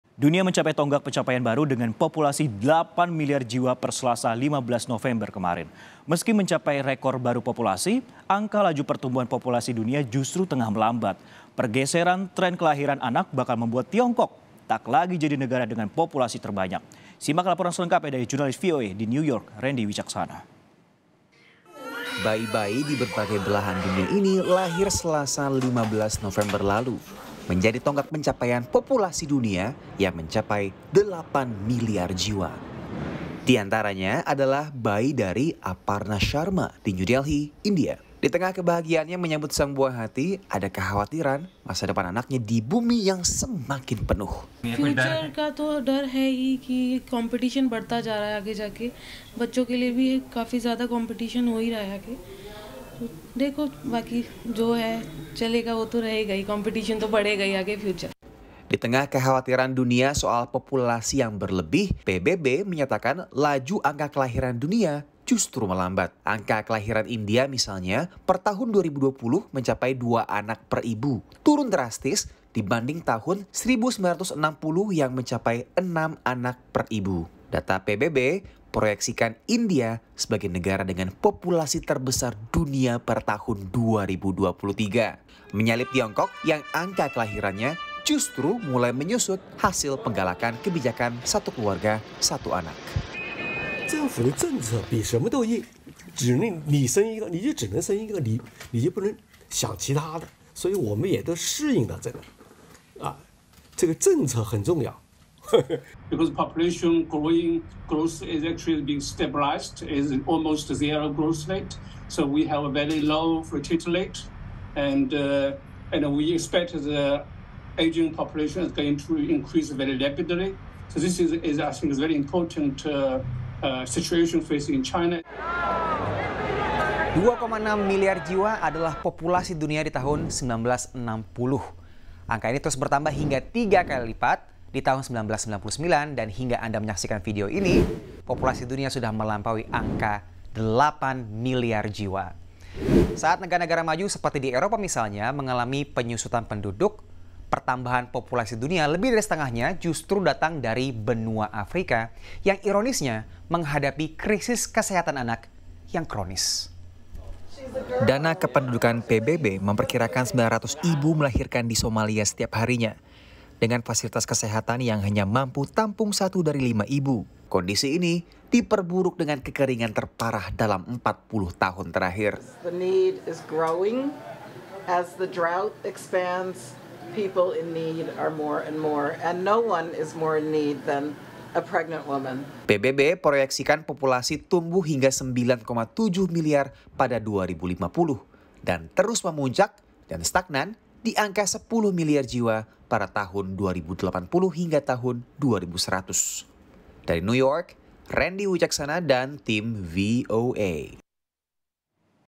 India Kejar Tiongkok Jadi Negara dengan Populasi Terbanyak! Bagaimana Bisa?
KOMPAS.TV - Dunia mencapai tonggak pencapaian baru dengan populasi 8 miliar jiwa per Selasa (15/11) kemarin.

Meski mencapai rekor baru populasi, angka laju pertumbuhan populasi dunia justru tengah melambat.

Pergeseran tren kelahiran anak bakal membuat Tiongkok tak lagi jadi negara dengan populasi terbanyak.

Lantas, siapa yang akan menggantikan Tiongkok jadi negara dengan populasi terpadat?

Apakah melambatnya pertumbuhan populasi dunia disebabkan oleh kewaspadaan soal perubahan iklim?

Atau karena hal lain?

Simak laporan selengkapnya dari Jurnalis VOA di New York, Rendy Wicaksana.